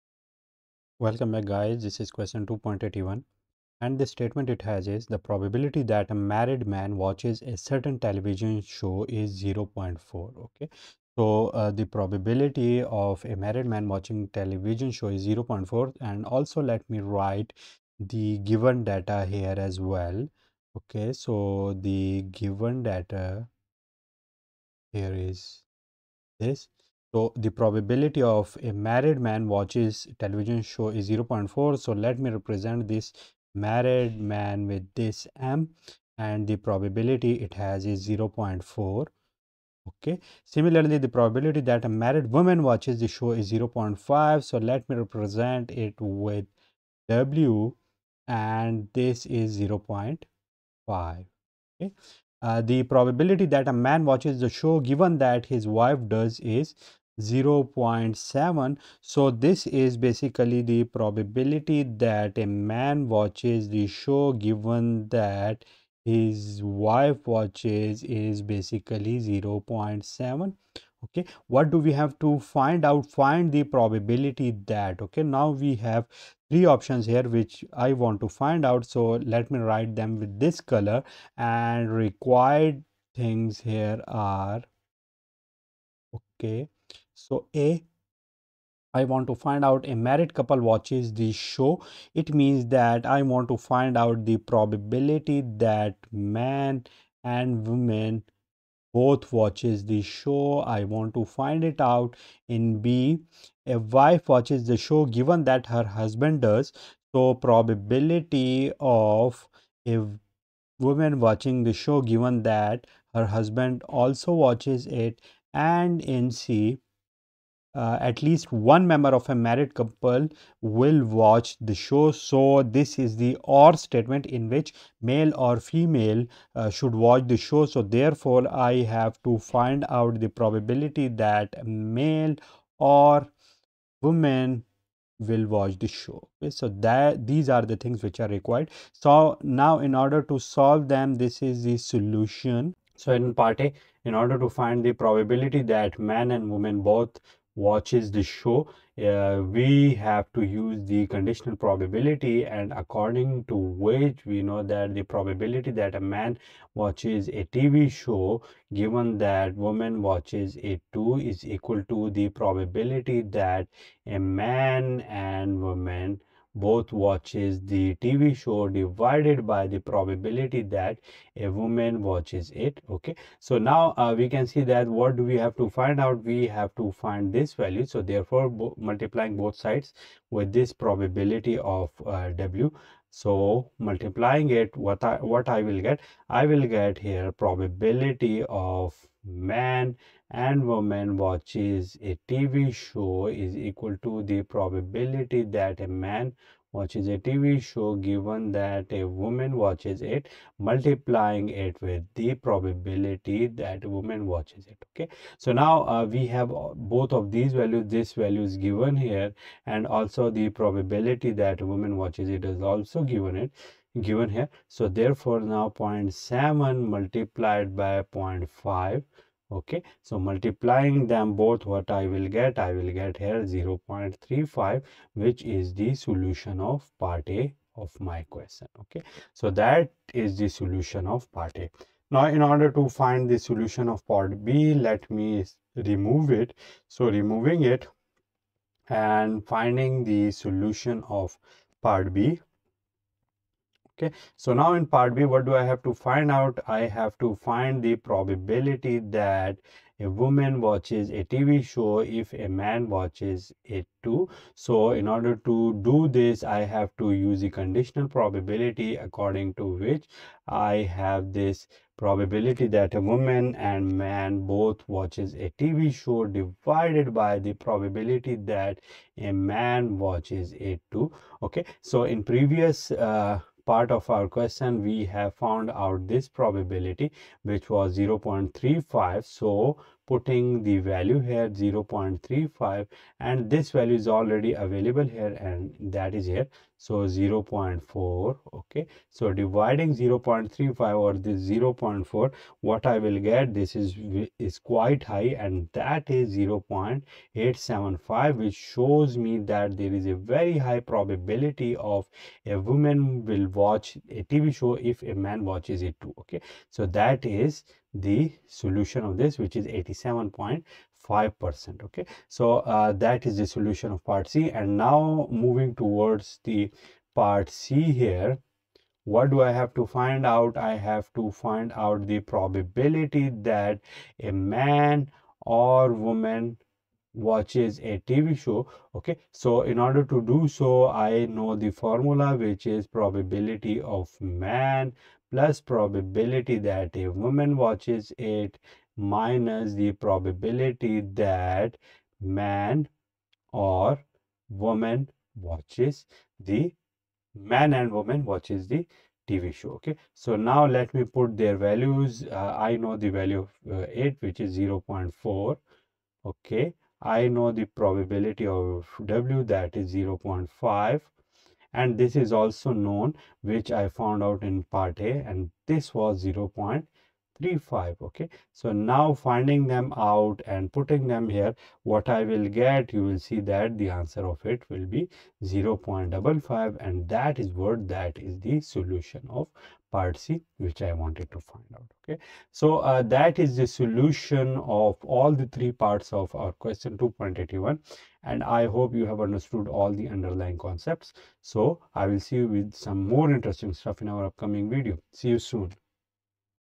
Probability & Statistics for Engineers & Scientists by Walpole | Solution Chap 2
This problem is related to conditional probability, which targets the solution of a problem involving two or more events interrelated with each other

2.81 The probability that a married man watches a certain television show is 0.4, and the probability that a married woman watches the show is 0.5. The probability that a man watches the show, given that his wife does, is 0.7. Find the probability that
(a) a married couple watches the show;
(b) a wife watches the show, given that her husband does;
(c) at least one member of a married couple will watch the show.

Happy Learning :-)